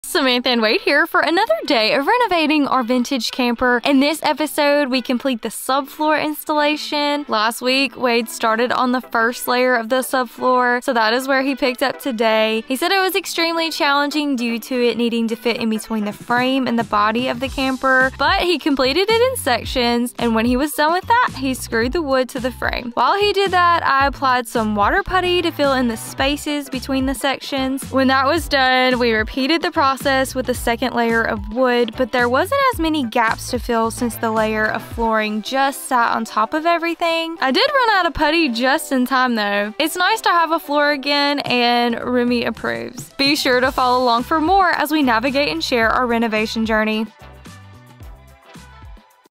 Vintage Camper Renovation Episode 20
This is our 1961 vintage Avion camper. Follow along for more as we remodel our new travel home.